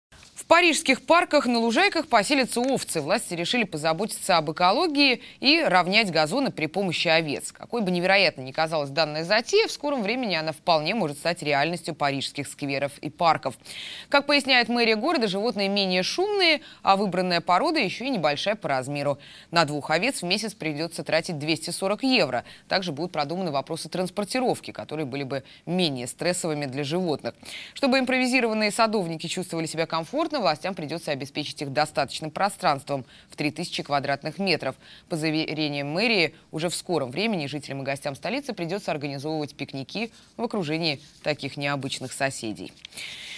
Парижские парки примут на работу овец
В Парижских парках на лужайках поселятся овцы. Власти решили позаботиться об экологии и решили ровнять газоны при помощи овец. Какой бы невероятной не казалась данная затея, в скором времени она вполне может стать реальностью парижских скверов и парков. Как поясняет мэрия города, животные менее шумные, а выбранная порода еще и небольшая по размерам. На двух овец, в месяц придется тратить 240 евро